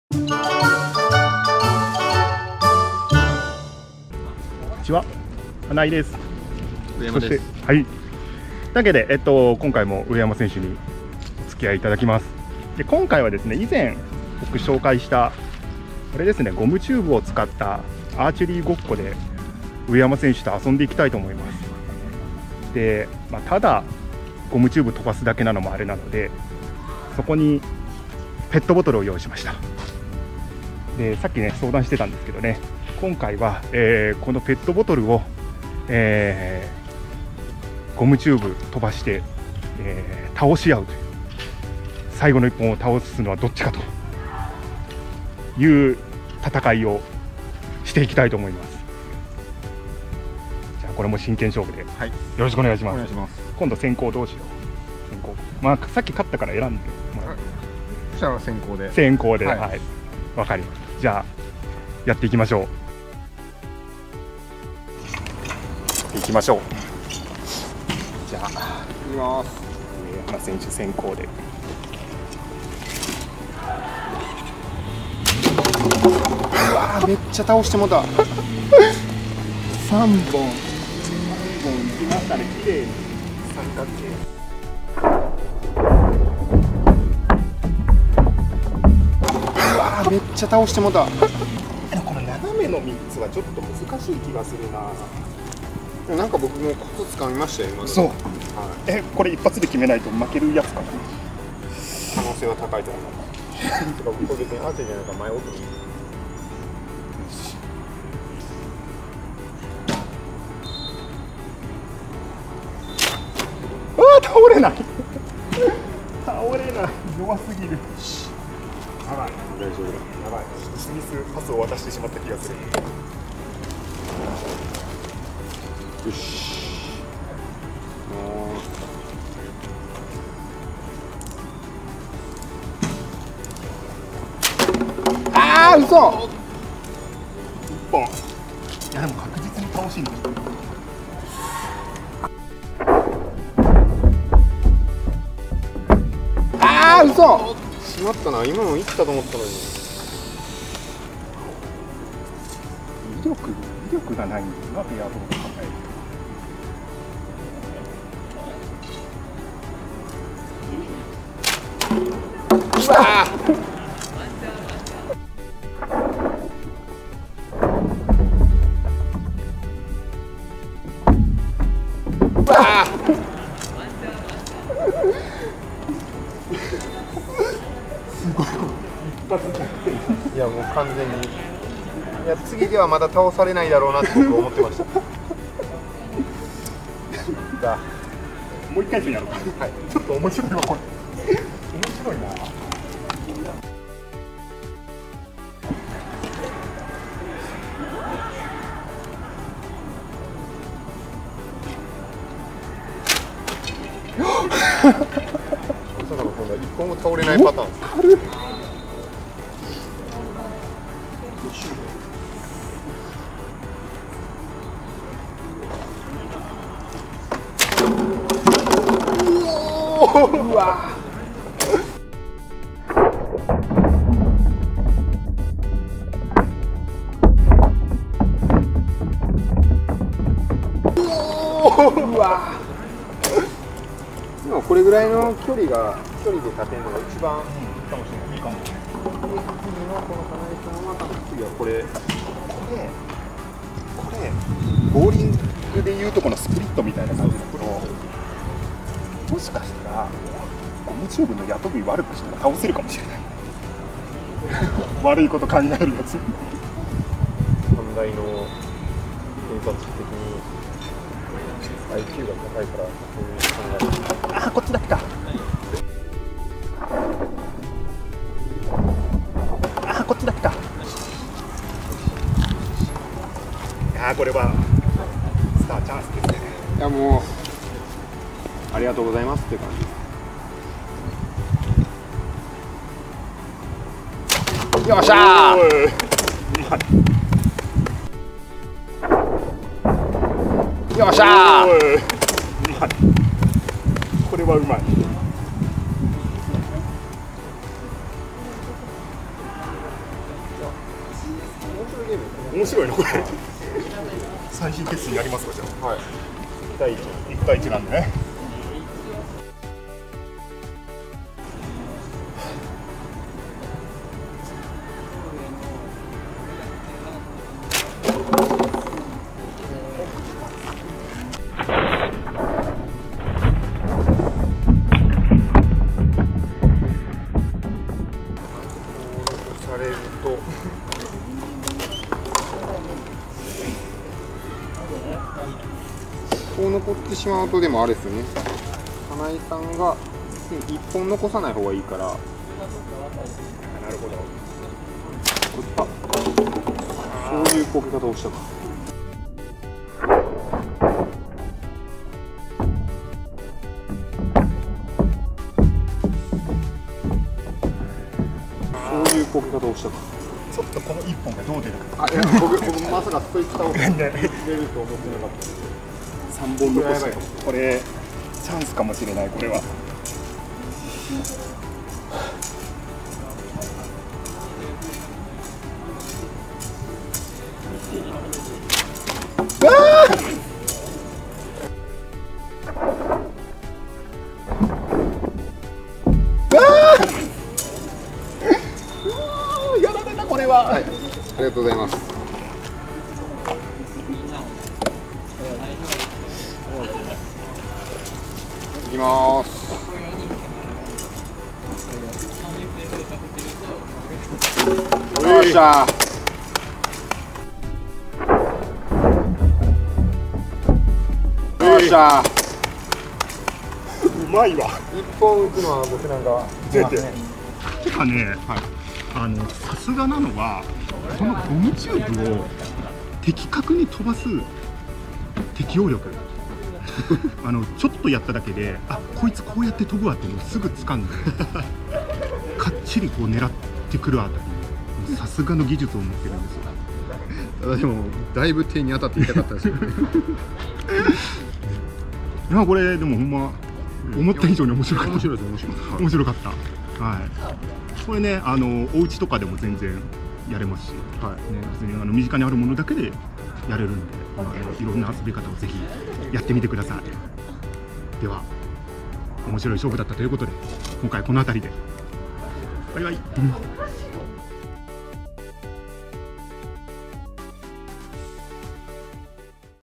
【アーチェリー】車いすでも問題なし！！スターと一緒にアーチェリーごっこ！！【アーチェリーごっこ】
ちはっ。今回は、スターと一緒にアーチェリーごっこで遊んでみました。
大人2人が大はしゃぎしてます。